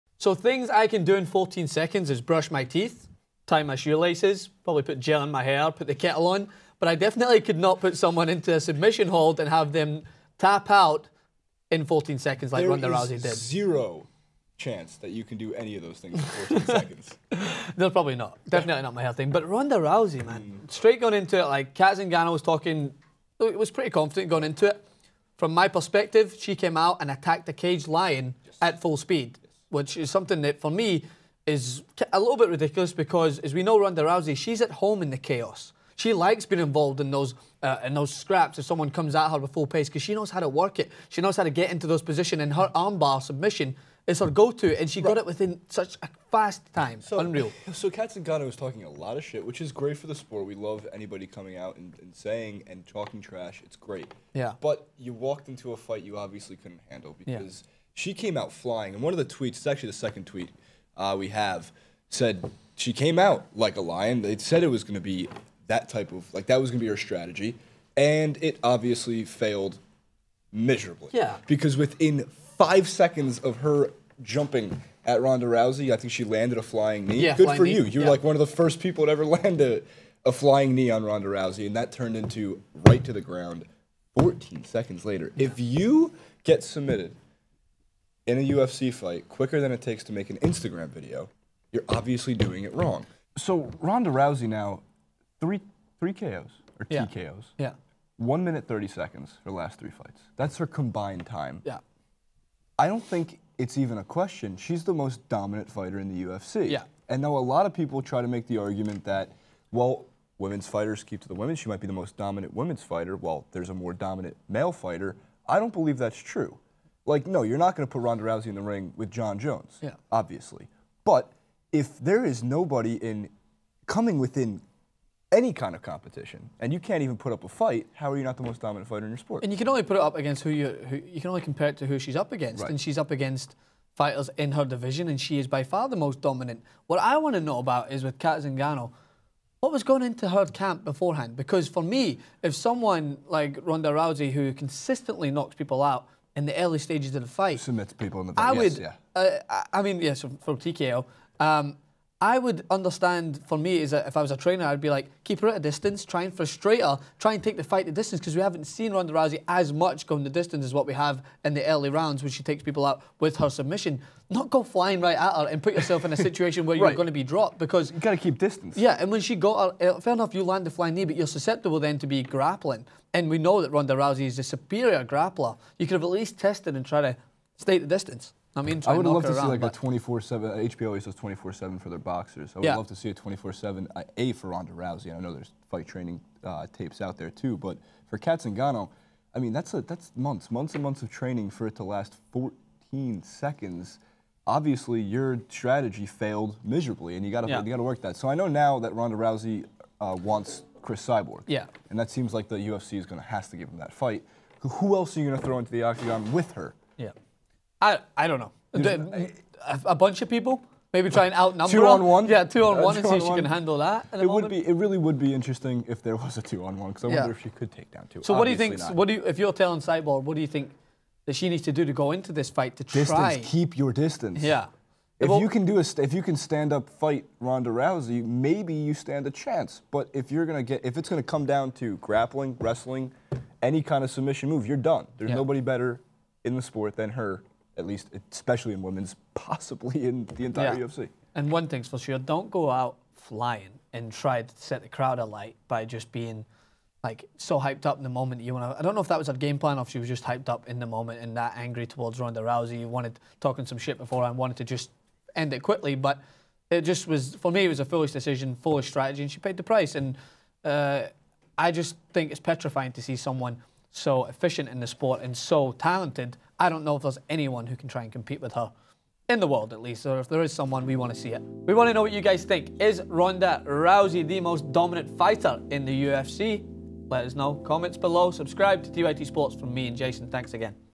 Ronda Rousey Unstoppable? | 14 Second Submission
Ronda Rousey has been widely accepted as one of the best UFC fighters and her 14 second submission against Cat Zingano made the record books. Rousey is the most fearsome women fighter the sport has seen and although Zingano came out with a strong flying knee, it only took ten additional seconds for Rousey to get her down the ground and tap out.